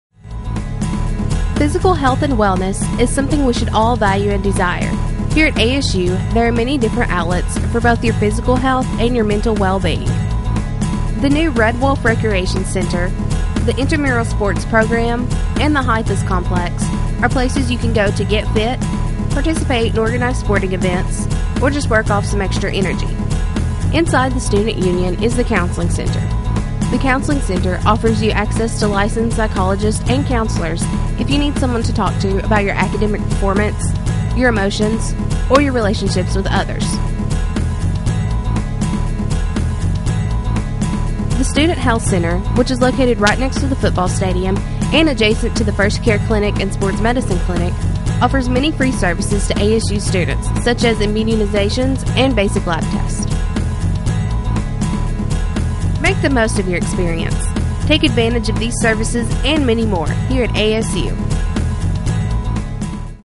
Health and Wellness
This video describes the Health and Wellness opportunities at Arkansas State University.